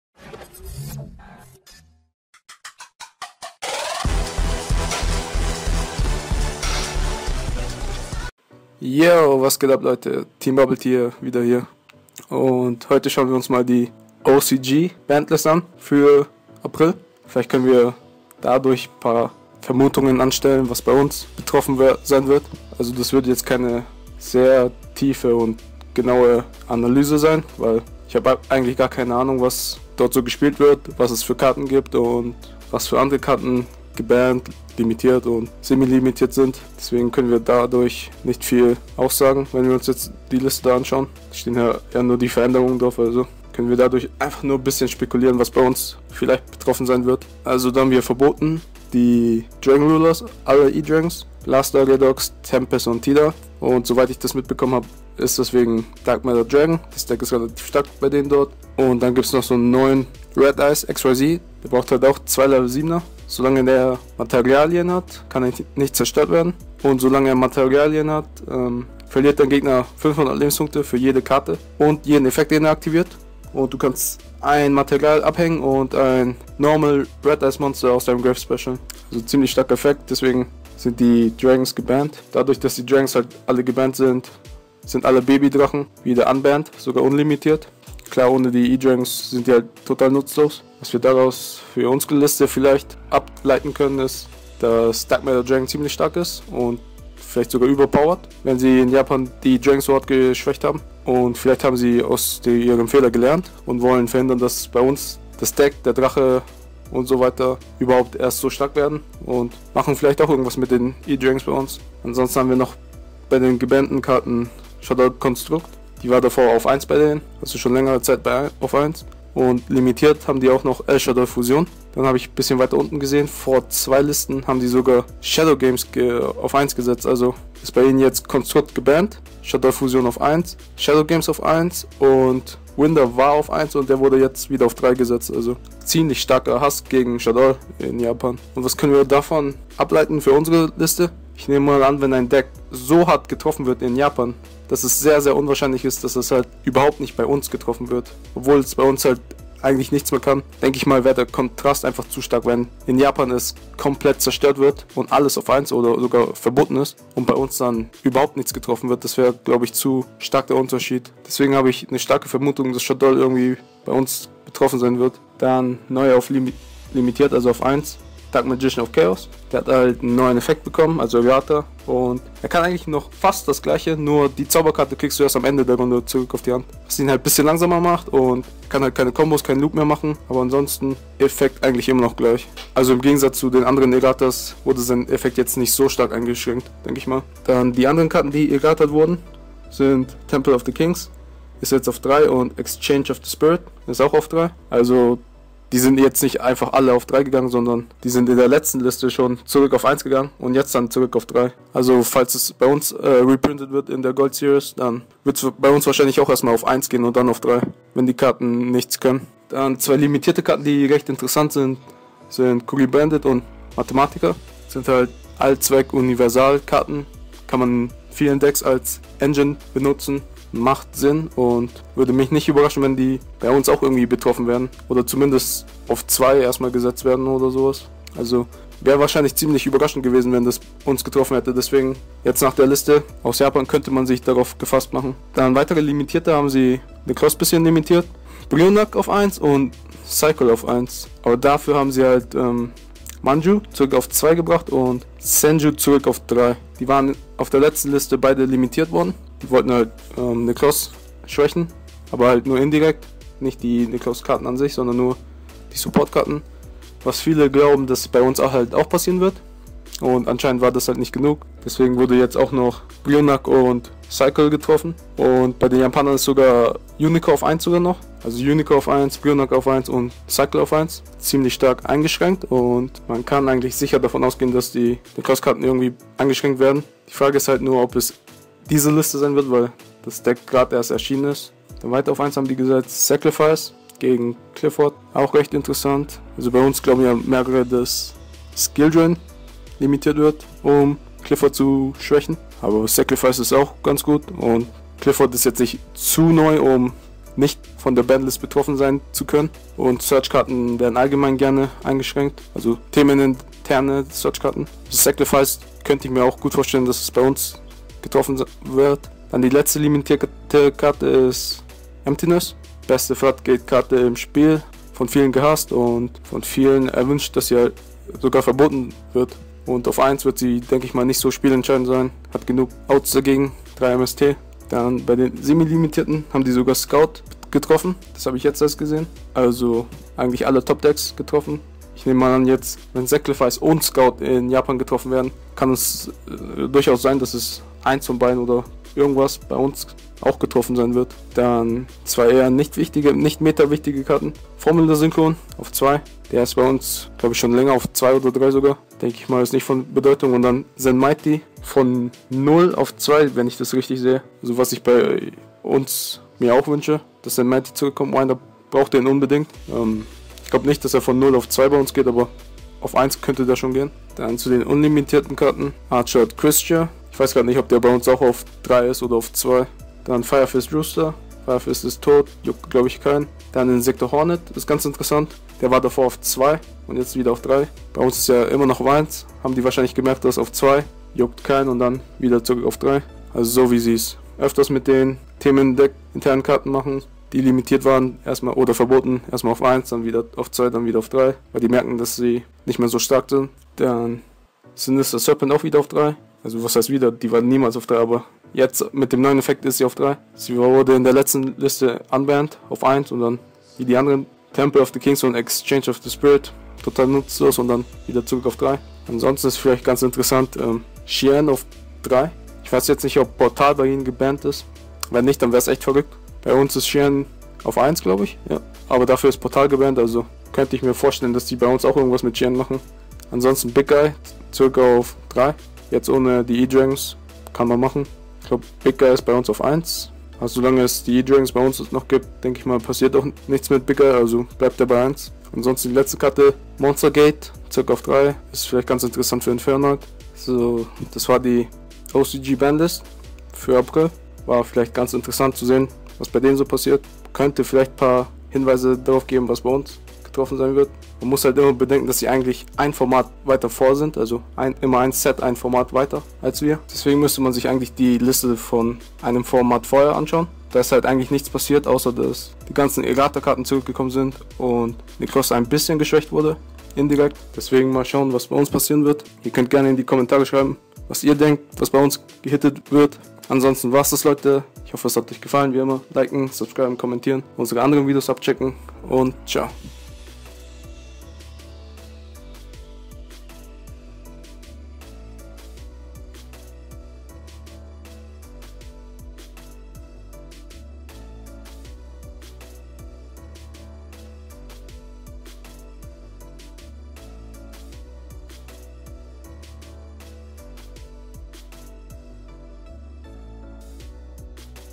TBT: OCG Ban List April 2015 Review Yu-Gi-Oh! (German/Deutsch)
OCG Ban Liste April 2015 Review
was haltet ihr von der Liste?
können wir etwas davon für unsere Ban Liste ableiten?
Unsere App:
unser Blog:
unser TCGMarket Account:
unsere Facebook Seite:
►DevPro: BadBoyBudha
►Dueling Network: KidRainen
►etcg: OMGTeamBubbleT
►Hearthstone: BadBoyBudha#2639
-thanks